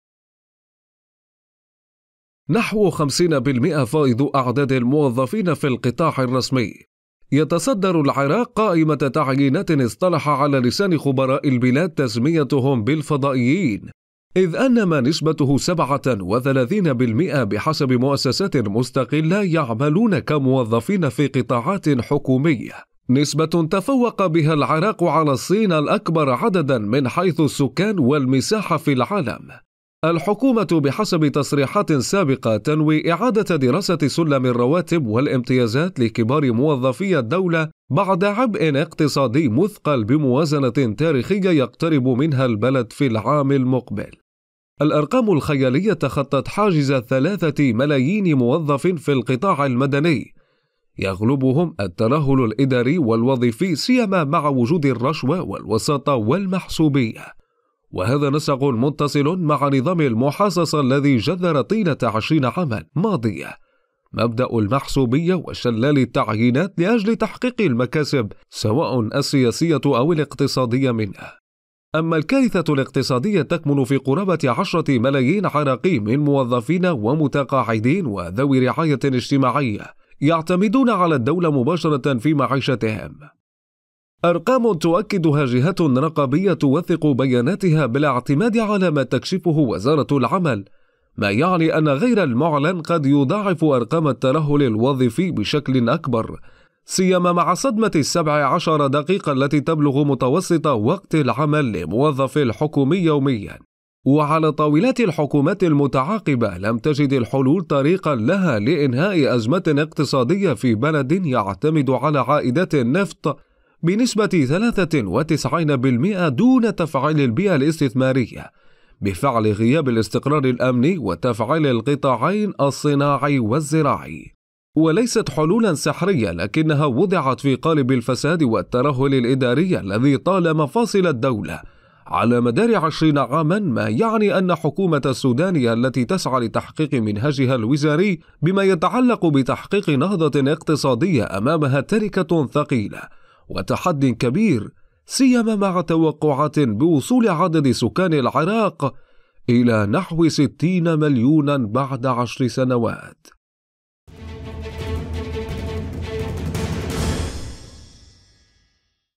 العراق يسجل رقما قياسيا لساعات عمل الموظفين ورواتبهم | تقرير
اذا كبر التحديات التي تواجه المنهاج الوزاري تكمن في علاج الترهل الوظيفي المتجذر في البلاد منذ نحو 20 عاما ماضية، والتي وضعت البلاد في مقدمة أكبر الدول المثقلة بملايين الموظفين الفضائيين المرهقين لموازنة الدولة في ظل غياب بيئة استثمارية فاعلة وارتفاع كبير لنسب البطالة.

موقع قناة التغيير الفضائية الالكتروني